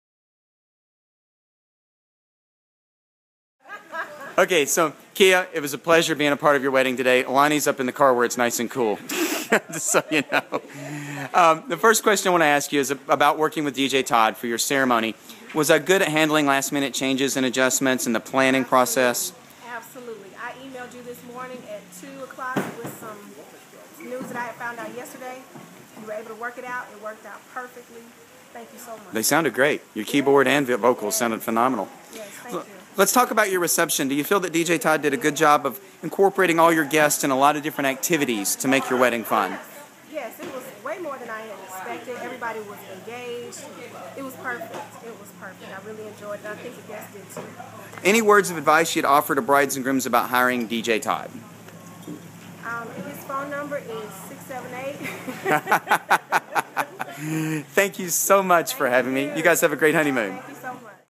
LaKia & Lonnie 7 28 12
LaKia & Lonnie talk about DJTOD for their wedding